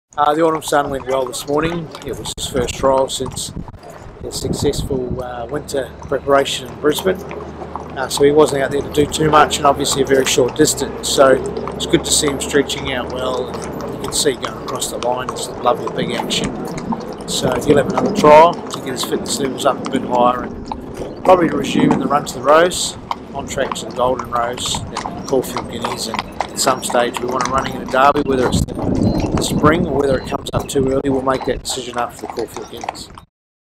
The Autumn Sun - Trial 14 August
Chris Waller chats to TDN AusNZ about The Autumn Sun after his trial at Randwick on 14th August.